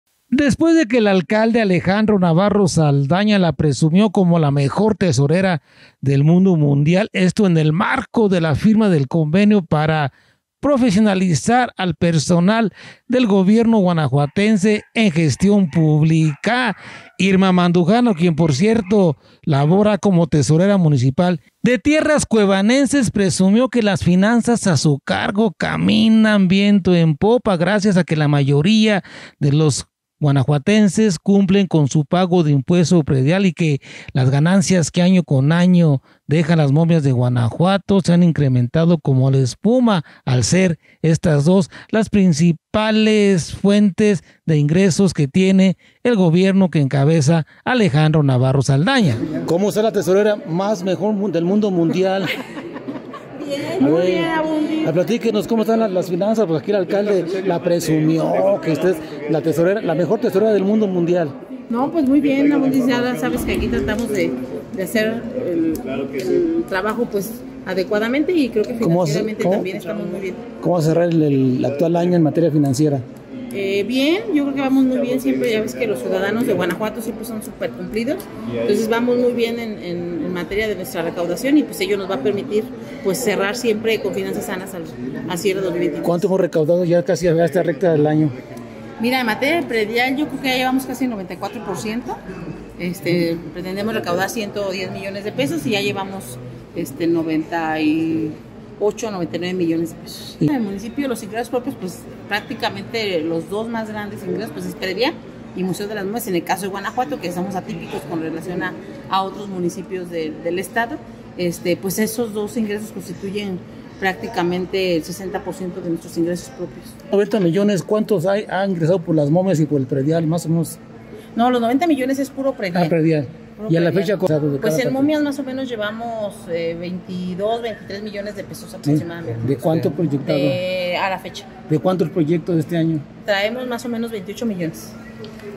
El impuesto predial sigue siendo los principales ingresos al gobierno cuevanense: Irma Mandujano
El impuesto predial, junto con las momias de Guanajuato, siguen siendo los principales ingresos al gobierno cuevanense: Irma Mandujano